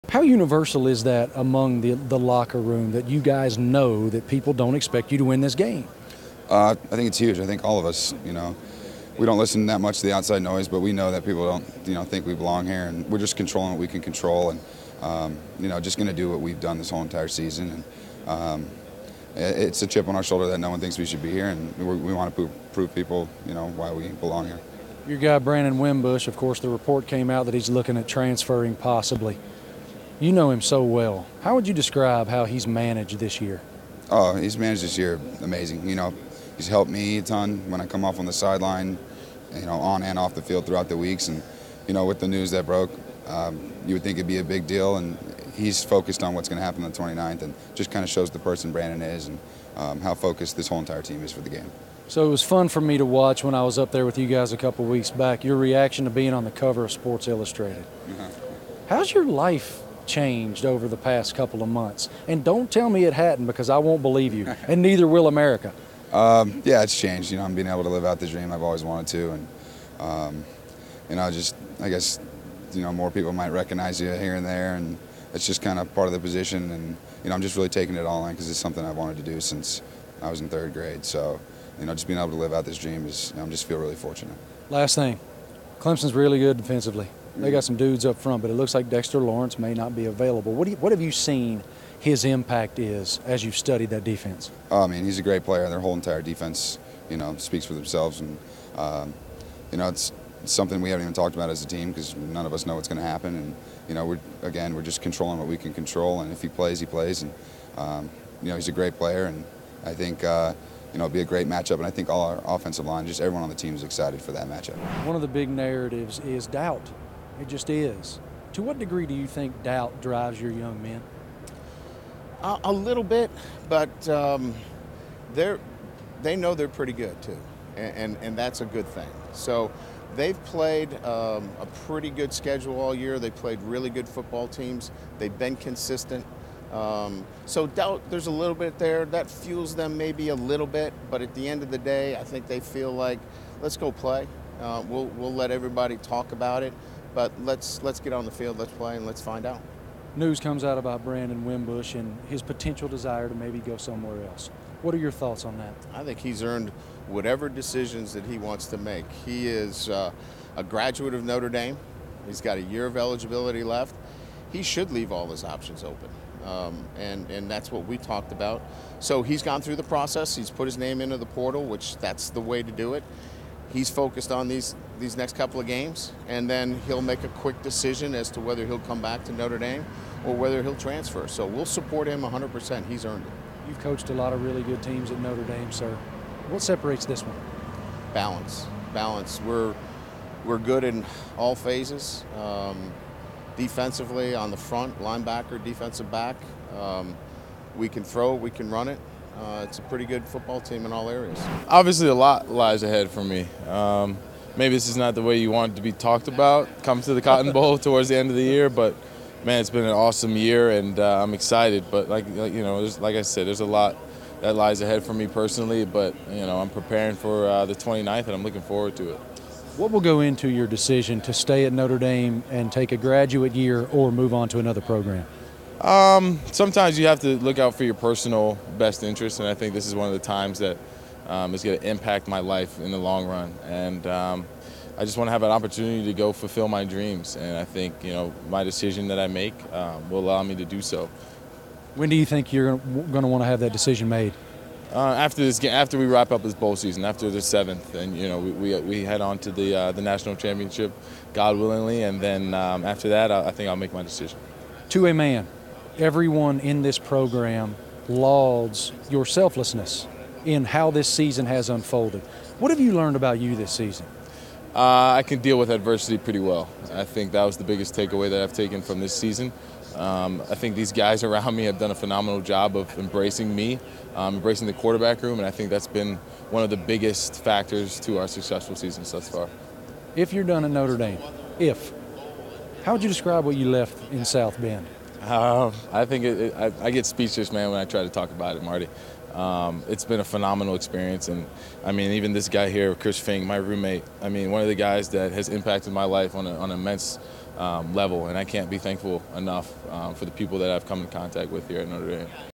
Notre Dame QB Ian Book: ‘We know people don’t think we belong here’ | College Football Playoff
Notre Dame QB Ian Book speaks to Marty Smith at the Cotton Bowl media day about having a chip on his shoulder, head coach Brian Kelly, the Fighting Irish's confidence and Brandon Wimbush understanding the significance of his transfer decision.